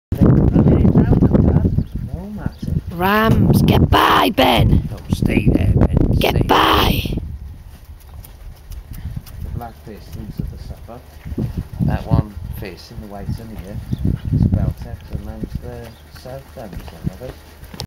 Rams and Sheep dog
Sheep dog Ben, having a well earned rest, while being watched by Rams.